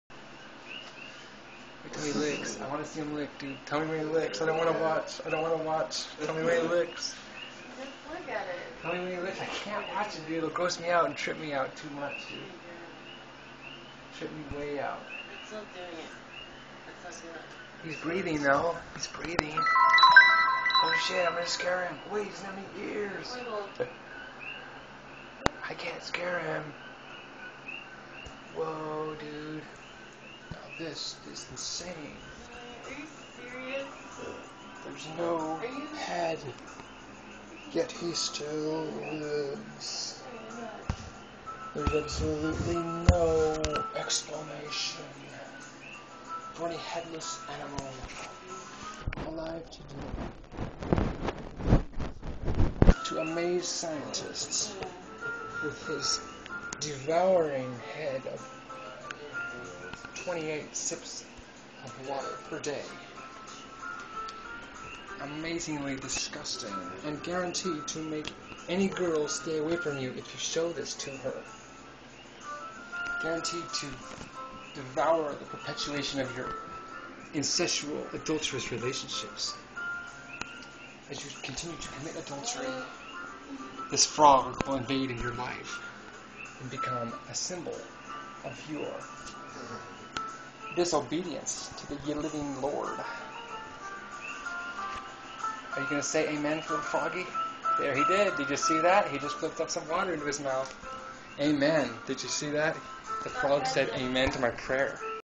Mutant headless frog licks the air
I finally found it!!! This is the headless frog video I thought I had lost forever. from back in Nov 2011...over 3 years ago.